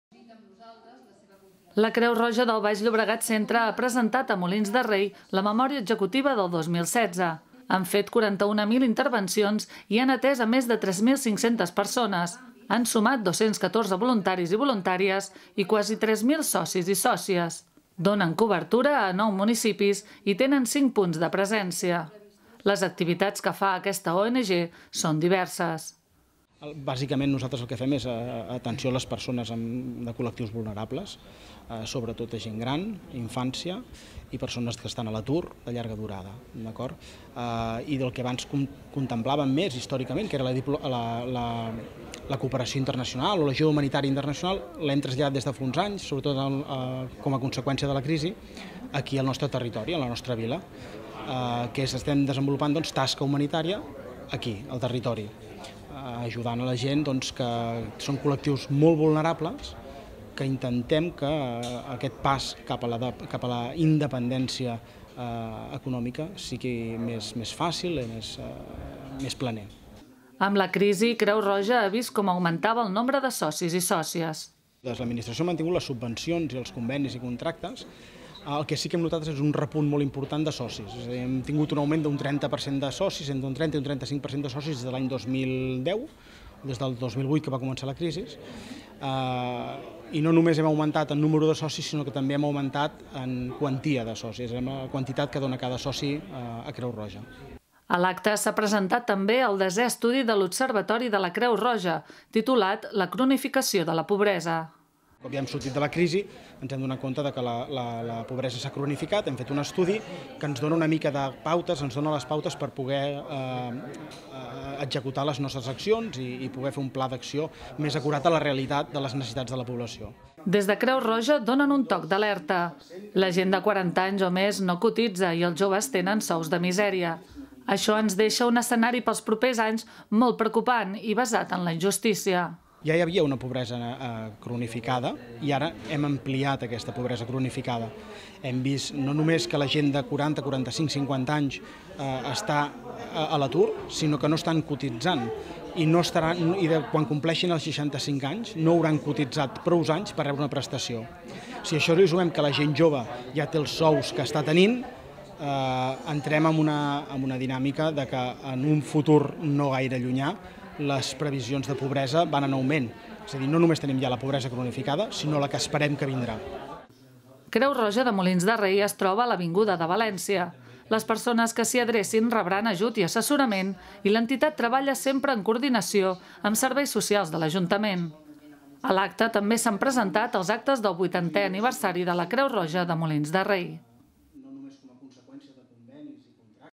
80 anys de Creu Roja Molins i Memòria 2016. 02-06-2017
Creu Roja alerta sobre la cronificació de la pobresa a la nostra societat en la presentació de la Memória Executiva de 2016